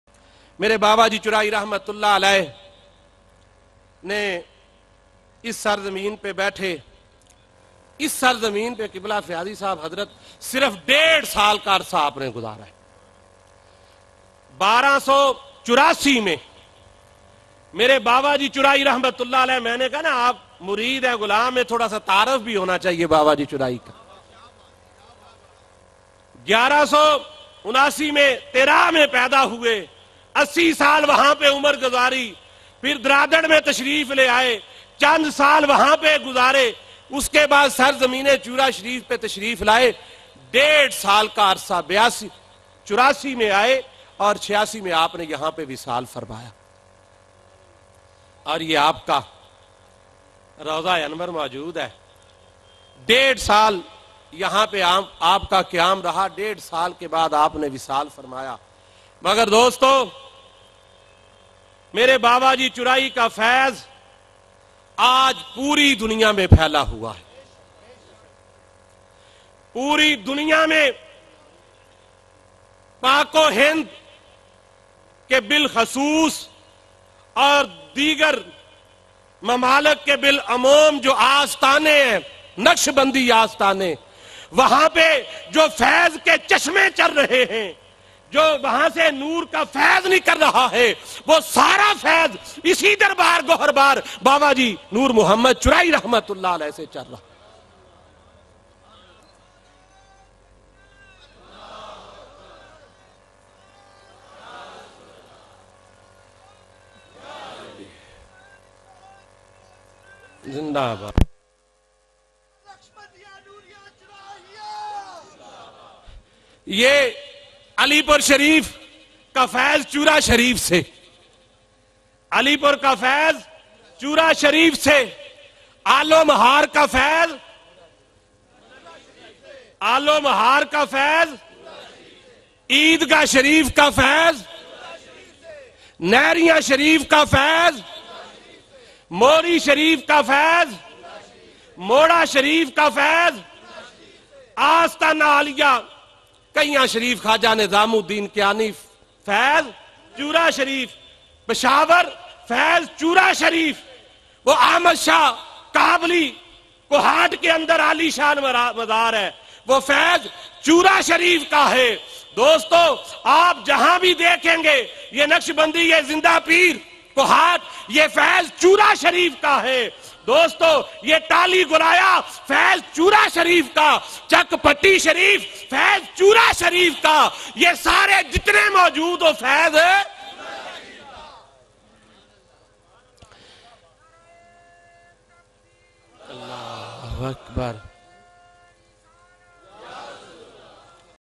Introduction Chura Shareef (Al-Hazrat Tahir Badshah Jee) by Maulana Ashraf Noori Sb
Introduction of Chura Shareef (Hazrat Peer Syed Muhammad Manzoor Asif Tahir Badshah Jee.. Sajjada Nasheen Markazi Dargaah Aastana Aalia Chura Shareef) by Maulana Ashraf Noori Sb (Recorded at Urs Mubarak 2009)